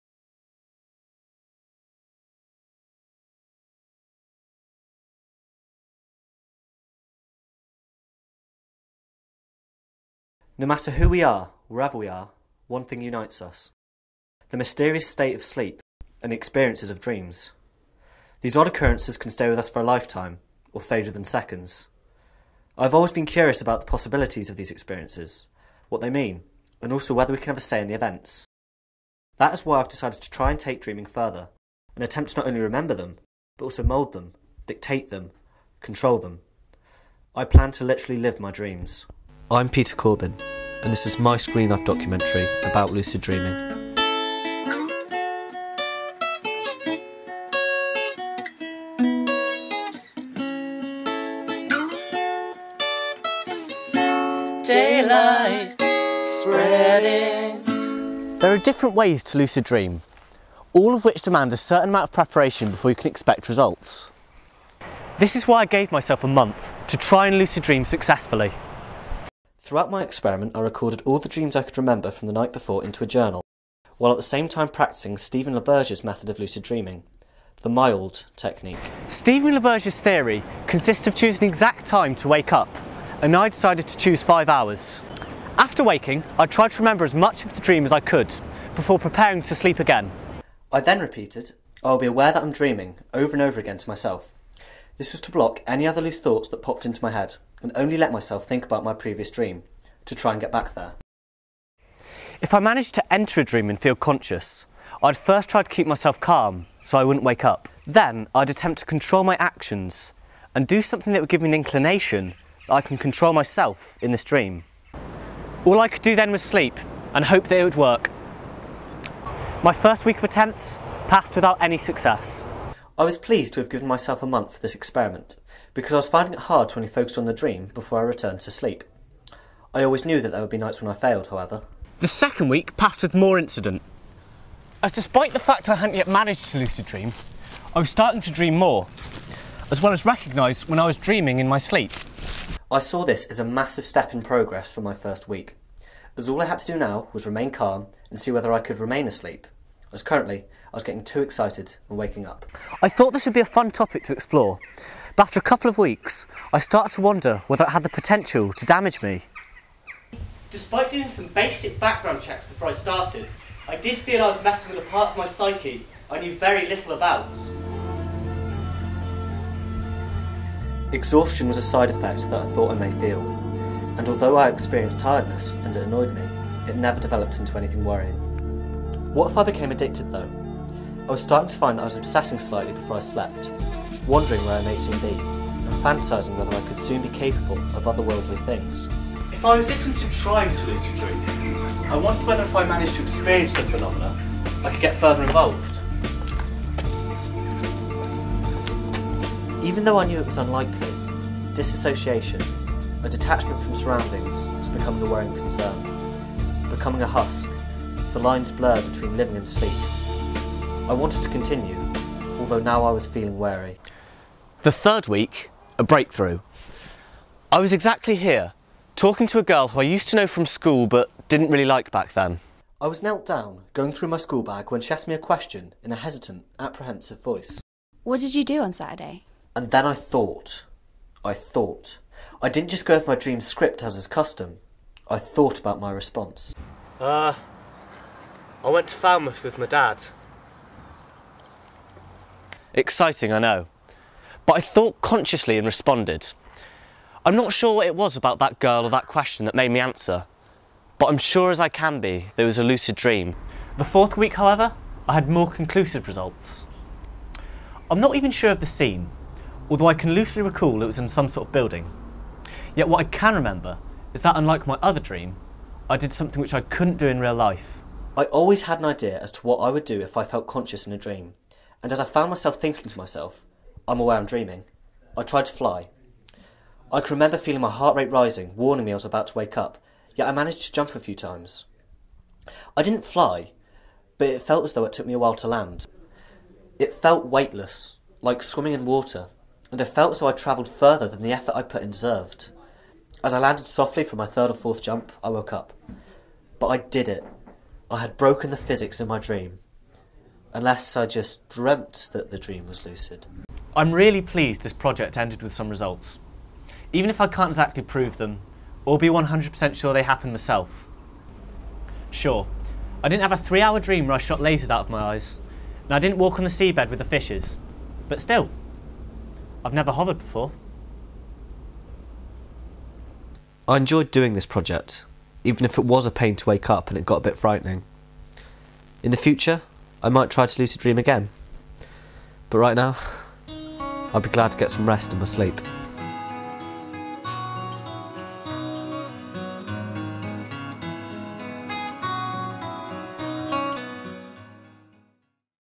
Peter - Screen Life
A 3-5 minute documentary about the lives/interests of the 1st year Extended Diploma Media students. The Screen Life project is the the first moving image assignment they complete whilst on the Extended Diploma course.
Pete looks in the the world of Lucid dreaming